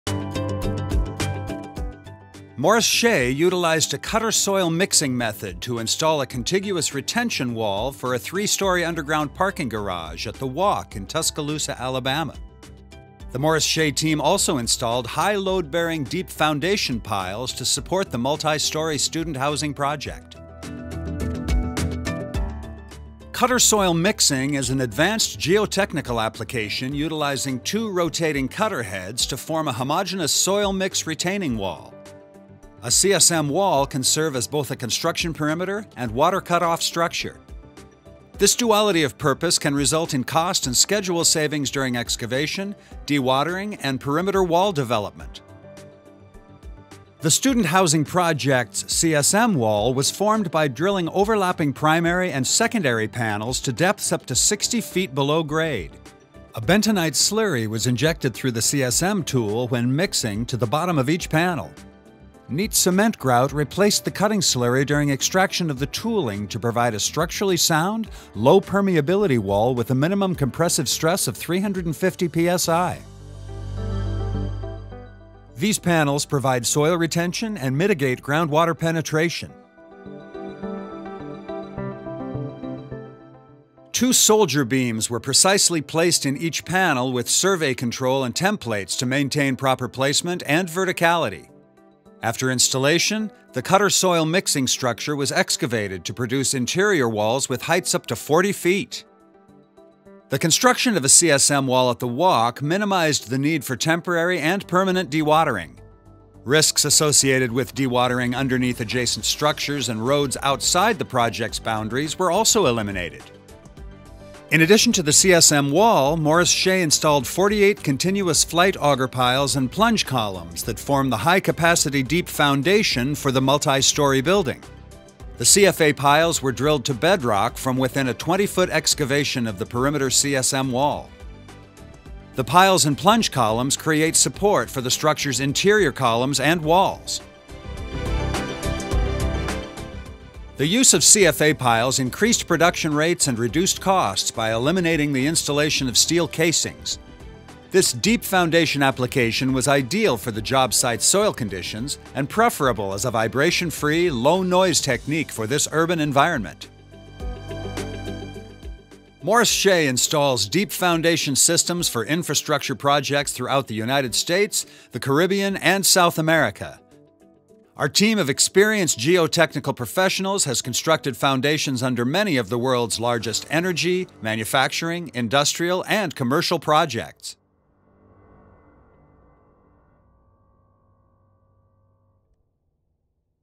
Morris-Shea Cutter Soil Mixing Wall & CFA Pile Deep Foundation: Tuscaloosa Student Housing Project
Morris-Shea utilized a Cutter Soil Mixing method to install a contiguous retention wall for a three story underground parking garage at The Walk in Tuscaloosa, Alabama. The Morris-Shea team also installed high load-bearing deep foundation piles to support the multi-story student housing project.

The student housing project’s CSM wall was formed by drilling overlapping primary and secondary panels to depths up to 60 feet below grade. These panels provide soil retention and mitigate groundwater penetration.

Morris-Shea cutter soil mixing is an advanced geotechnical application utilizing two rotating cutter heads to form a homogenous soil mix retaining wall. A CSM wall can serve as both a construction perimeter and water cutoff structure. This duality of purpose can result in cost and schedule savings during excavation, dewatering and perimeter wall development.